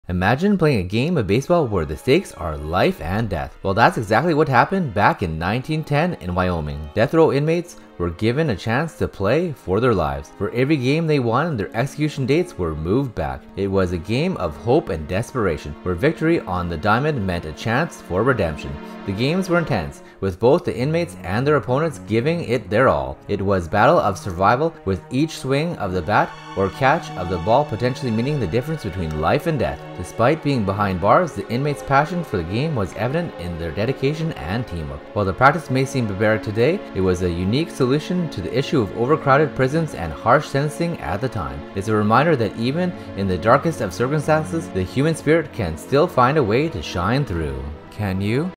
A Game of Life or Death Baseball
In this video, we explore a little-known part of American history that will leave you both shocked and inspired. Imagine a game of baseball where the stakes are life and death. Back in 1910 in Wyoming, that's exactly what happened. Death row inmates were given a chance to play for their lives. For every game they won, their execution dates were moved back.

This was a game of hope and desperation, where victory on the diamond meant a chance for redemption. The games were intense, with both the inmates and their opponents giving it their all. It was a battle for survival, with each swing of the bat or catch of the ball potentially meaning the difference between life and death.

Despite being behind bars, the inmates' passion for the game was evident in their dedication and teamwork. While the practice may seem barbaric today, it was a unique solution to the issue of overcrowded prisons and harsh sentencing at the time. It's a reminder that even in the darkest of circumstances, the human spirit can still find a way to shine through.

Join us as we delve into the fascinating and little-known history of this life-and-death baseball game. Discover the stories of the inmates who played, their opponents, and the social and political context that made this game possible. It's a story that will leave you amazed, moved, and inspired. So grab some popcorn and settle in for an unforgettable journey into the past.